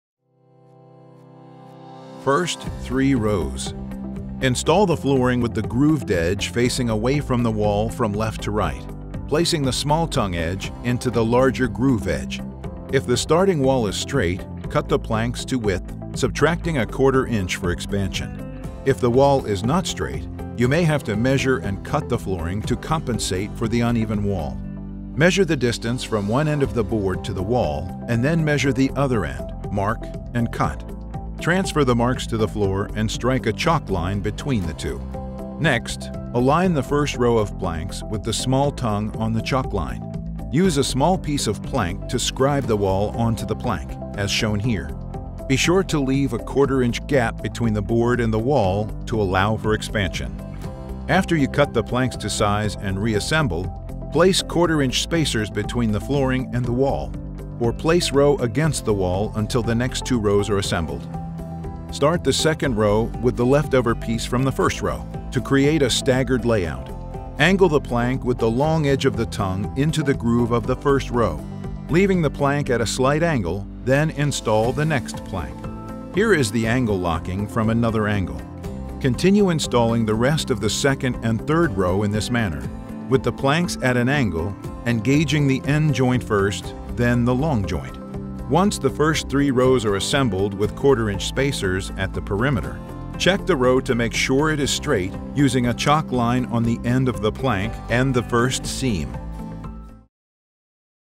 Laying the First Few Rows of Flooring | Tips and Tricks | Armstrong Flooring
Accurate installation of the first three rows of plank flooring is essential to every flooring project, even when the walls are uneven. This video will show you how to install the first three rows correctly.